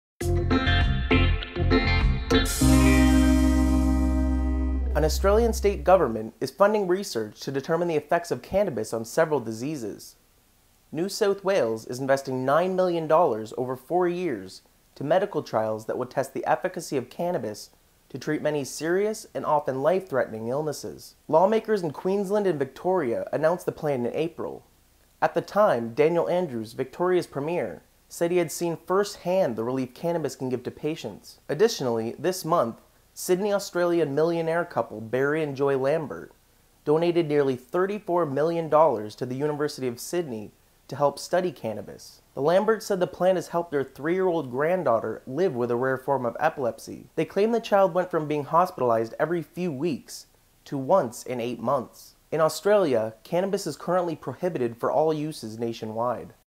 Australian State Funding Cannabis Studies
New South Wales, Australia is spending $9 million on cannabis research while a Sydney couple donated nearly $34 million for studies on the plant.